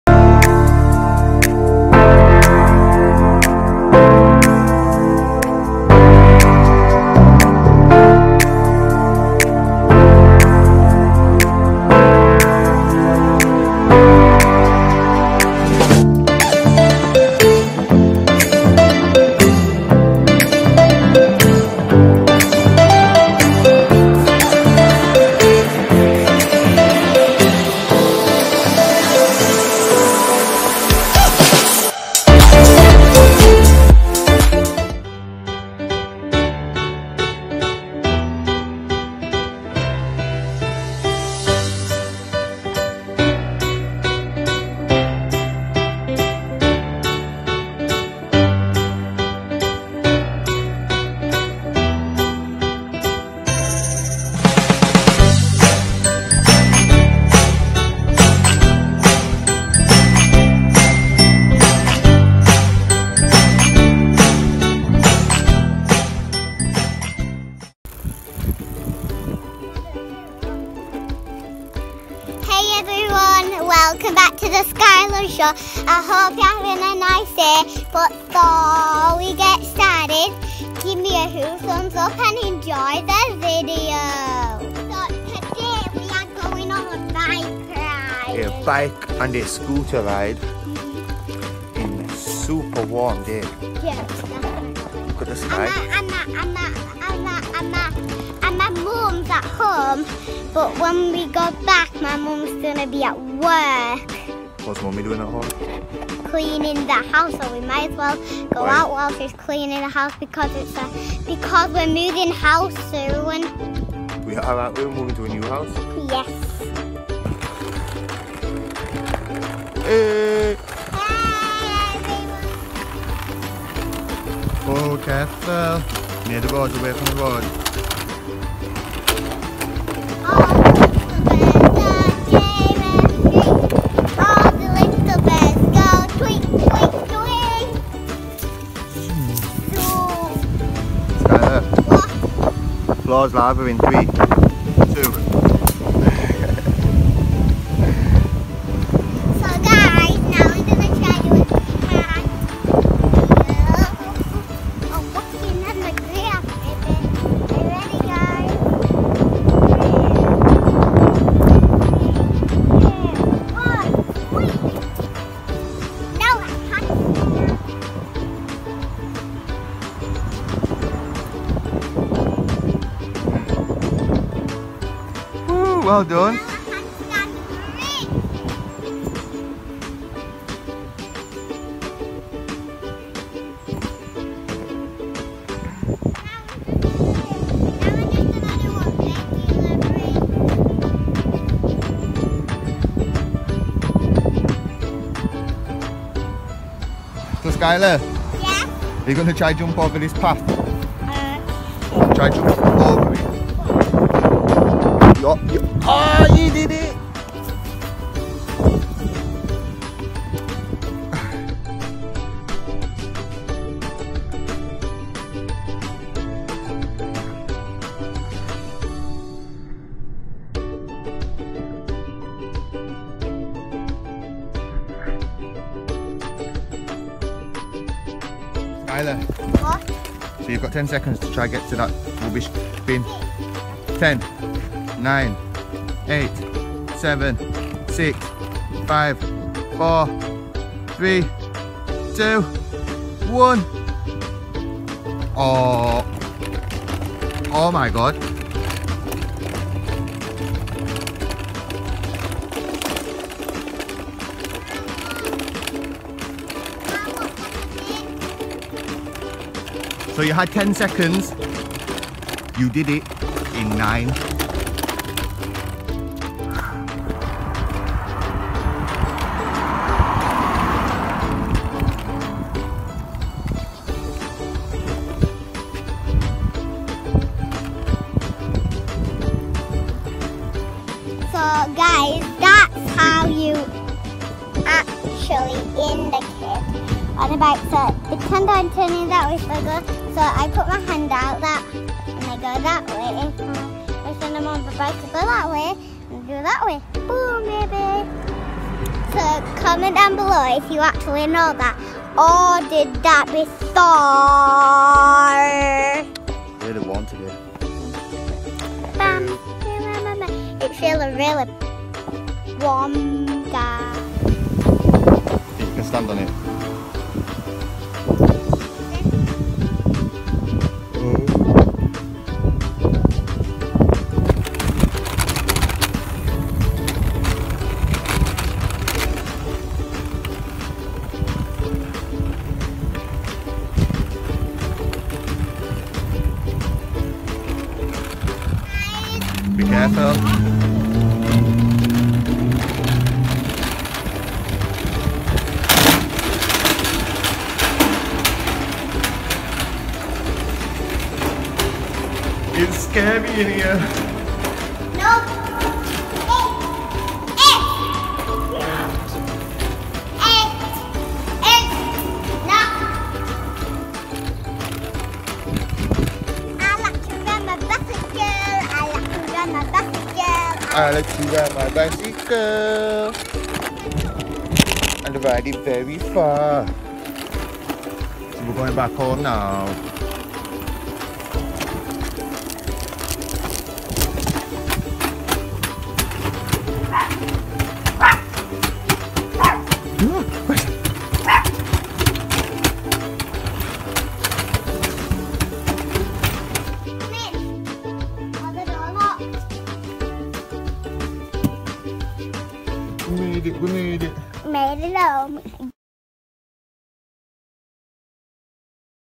Bike Ride On A Sunny Day!! Watch Skyla Go Super Fast!!
Come ride with skyla and have fun in the sun!! Skyla talks about how to indicate on a bike and shows off some of her gymnastic skills!!